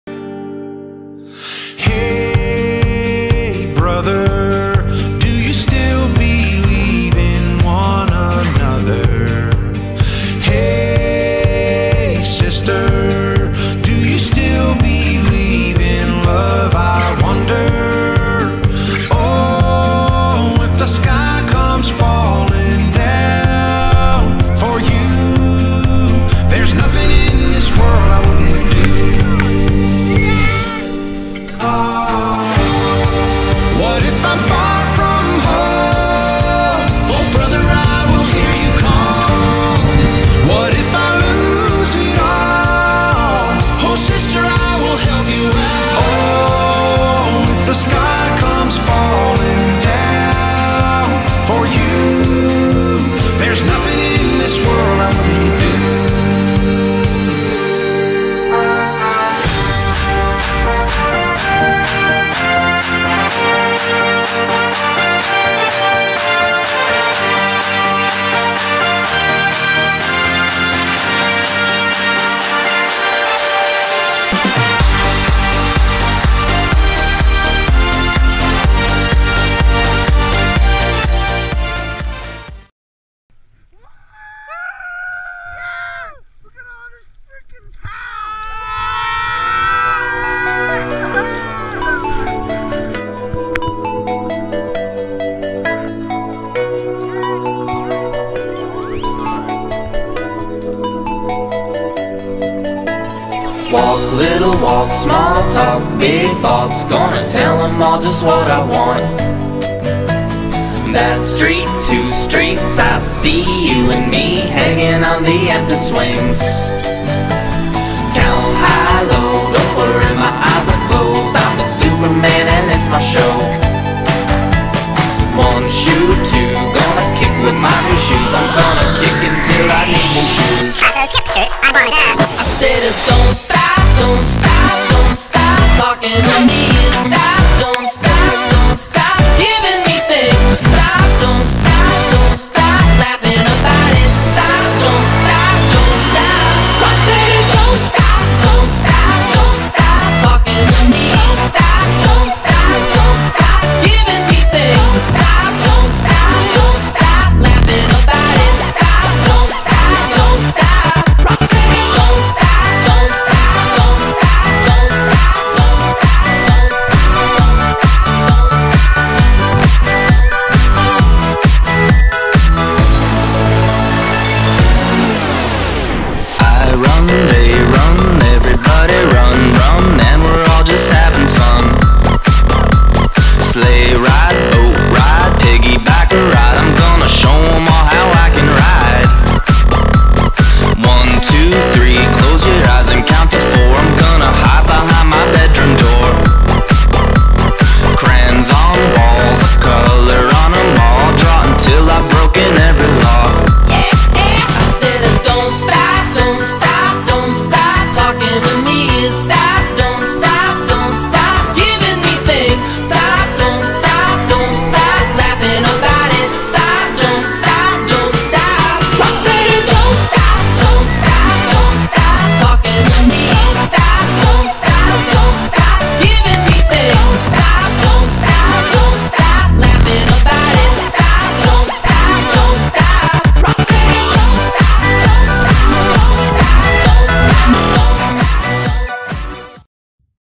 Shredding POW POW on White Crown at Breckenridge GoPro
Crazy good weekend with good people and phenomenal snow.